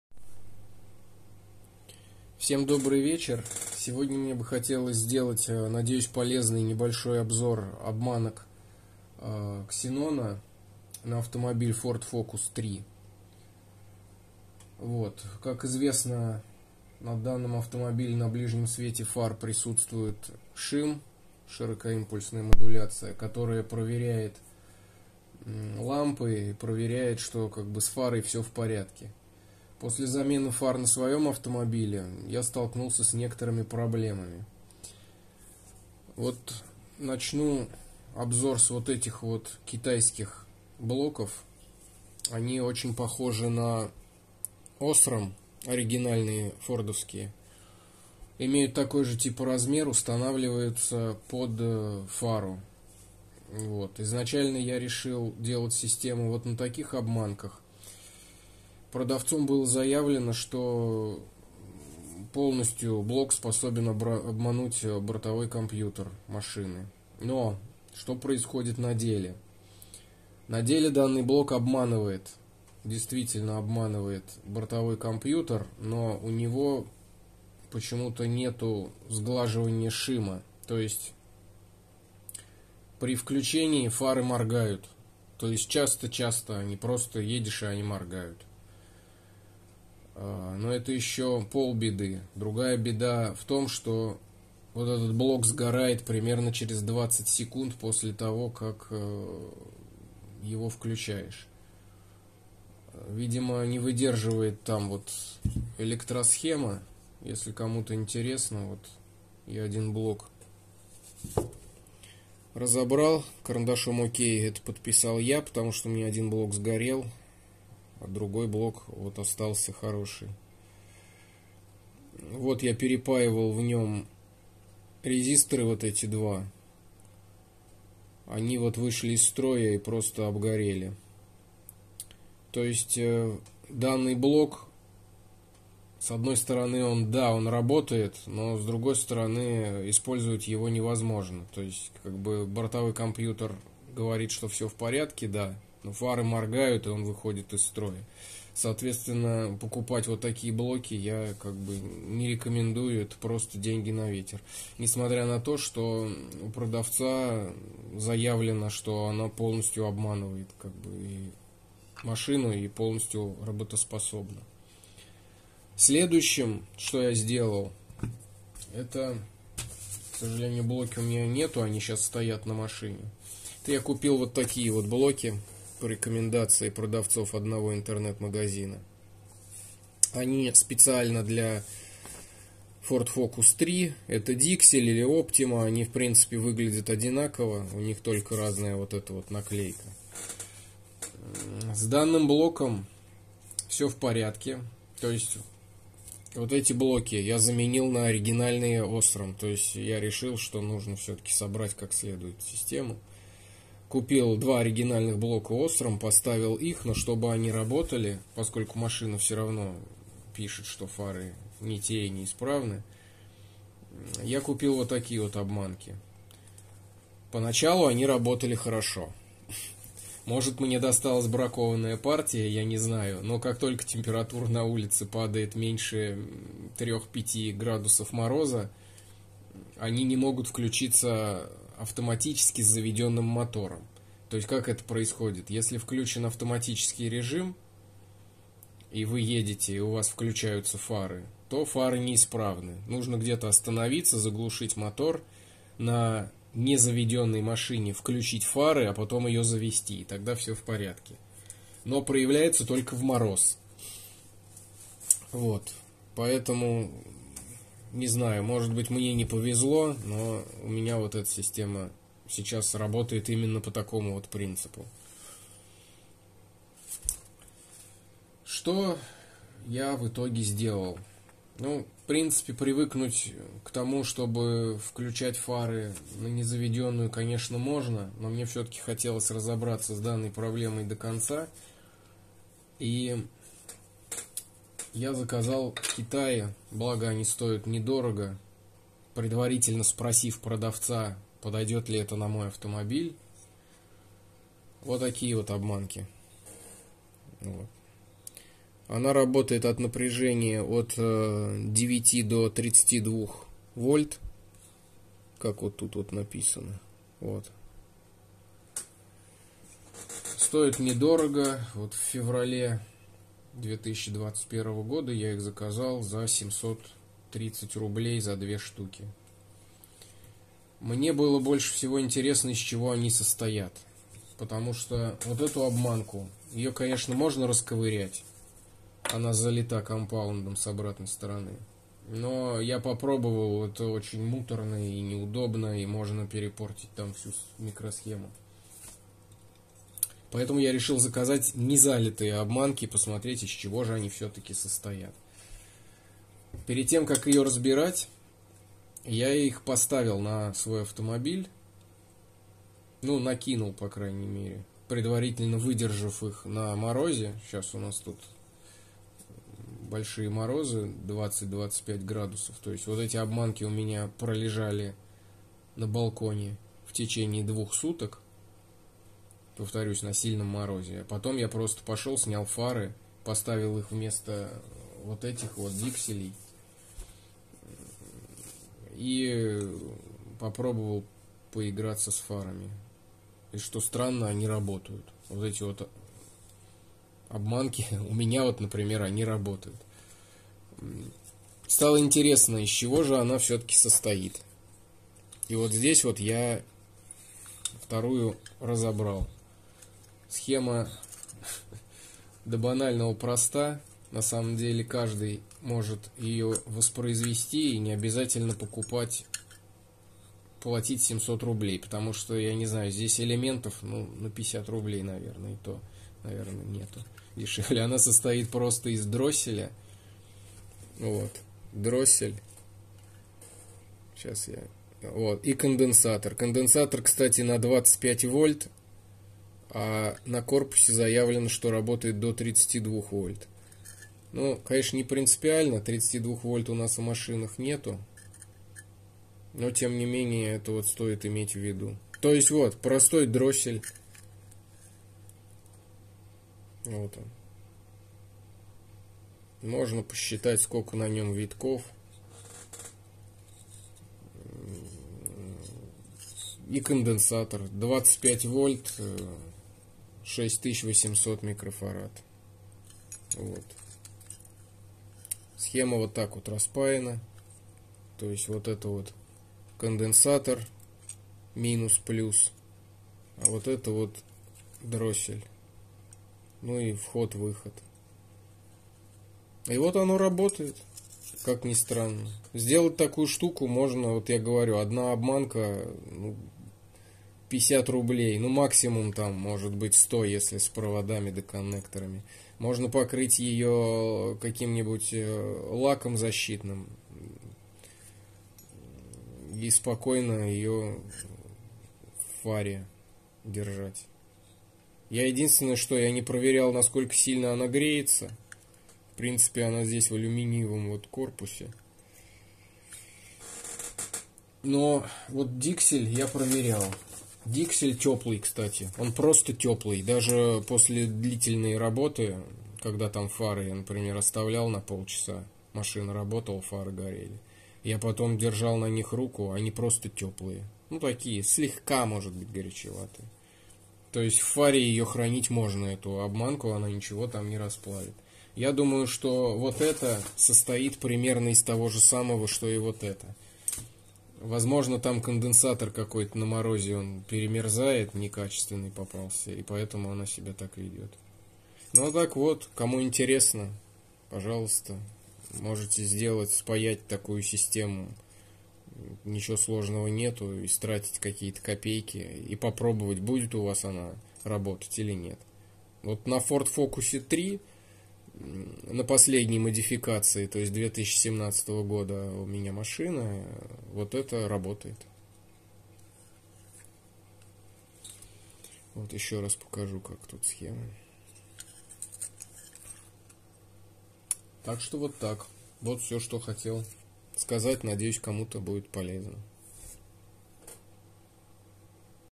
Виды и состав обманок ксенона на примере Ford Focus 3
Расскажу, какие виды обманок есть на Ford Focus 3, и как они работают на автомобиле, из чего состоят. Основано на собственном опыте.